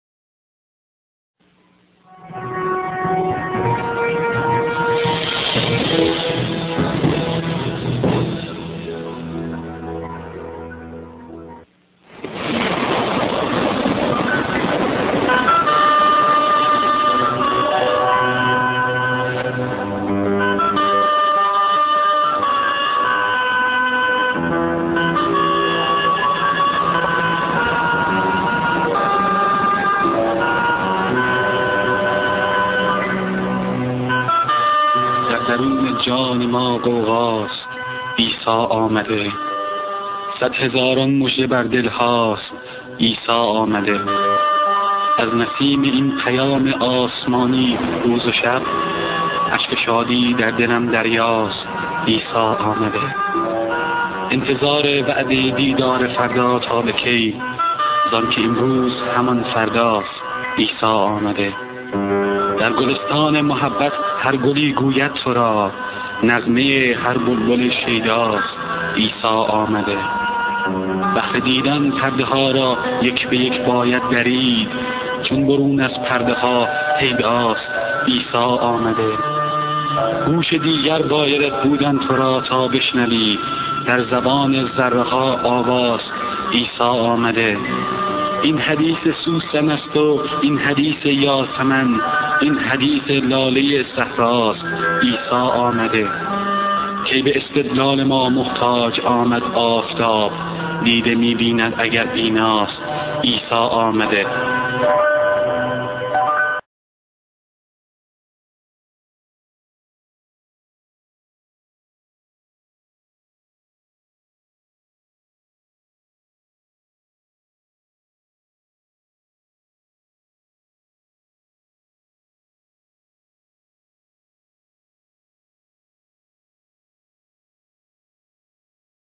Persian Poetry Isa Your Saviour Has Come - FarsiNet Poetry
Persian Poetry about Isa The Christ and Saviour by Geracheh Daqi - Farsi Christian Poetry 1000+ years of Persian poetry at FarsiNet